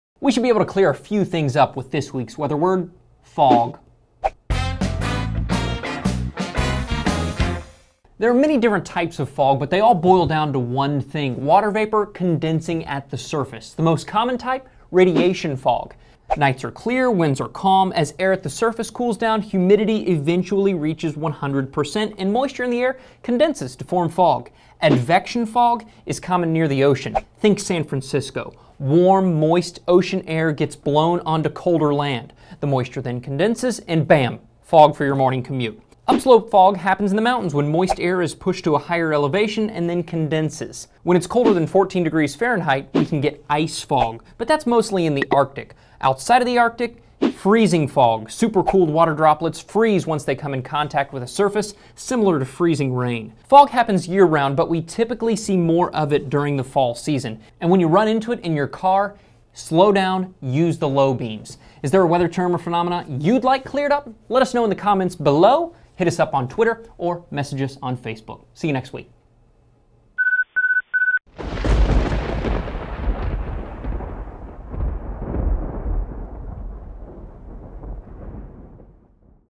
What is Fog? | Weather Word of the Week
We should be able to clear things up with this week's word: Fog

There are many different types of fog, but they all boil down to one thing: water vapor condensing at the surface.

The most common is radiation fog.
Nights are clear and winds are calm.
As air at the surface cools down, humidity eventually reaches 100%, and moisture in the air condenses to form fog.

Advection fog is common near the ocean - think San Francisco.
Warm, moist ocean air gets blown onto the colder land.
The moisture then condenses, and bam - fog for your morning commute.

Upslope fog happens in the mountains when moist air is pushed to a higher elevation and then condenses.

When it's colder than 14ºF, we can get ice fog. But more common outside of the Arctic is freezing fog - supercooled water droplets freeze once they come in contact with a surface, similar to freezing rain.

Fog happens year-round, but we typically see more of it during the Fall season.

And when you run into it in your car, slow down and use the low beams.

See you in a week!